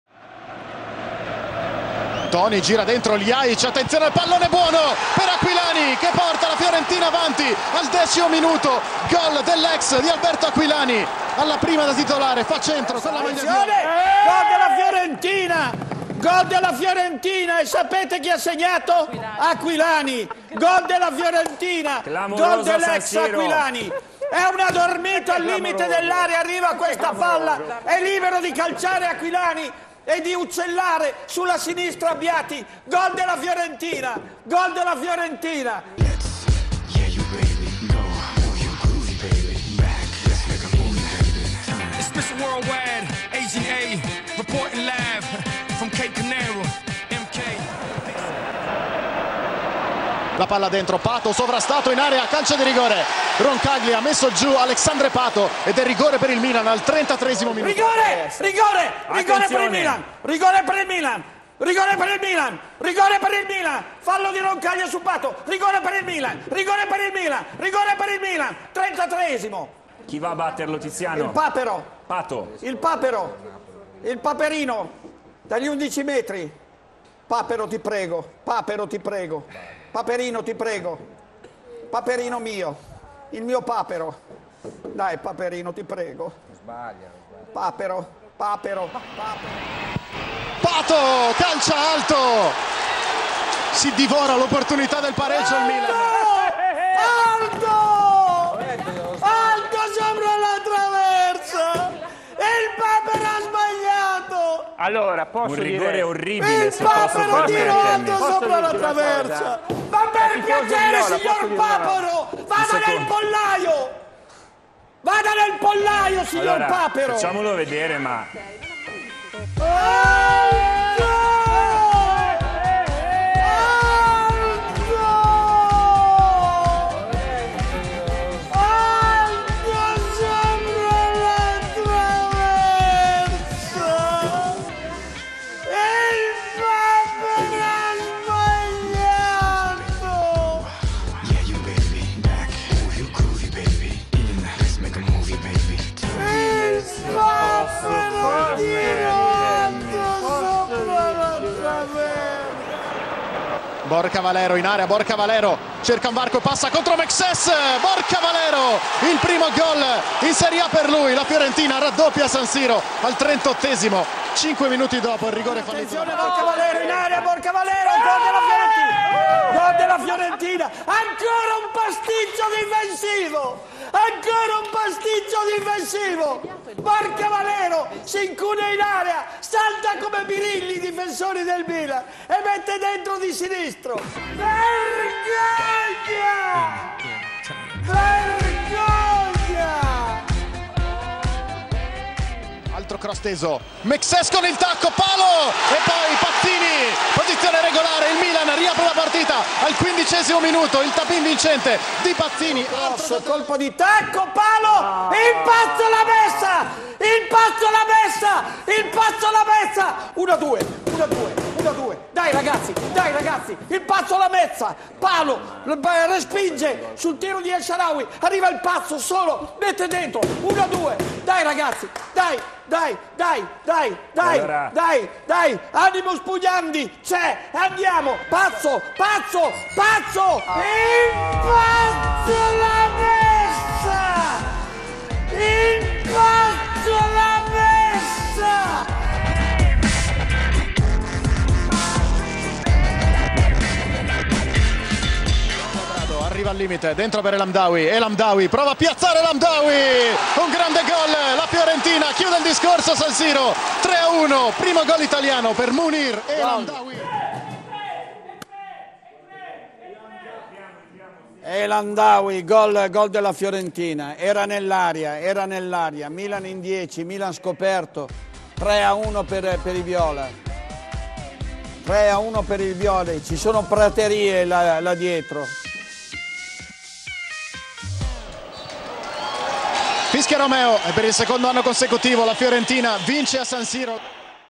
Milan - Fiorentina 1-3 - La disfatta Milanista del 12-11-2012.
Dodicesima giornata del Campionato di Serie A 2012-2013. La Fiorentina batte il Milan a San Siro per il secondo anno consecutivo.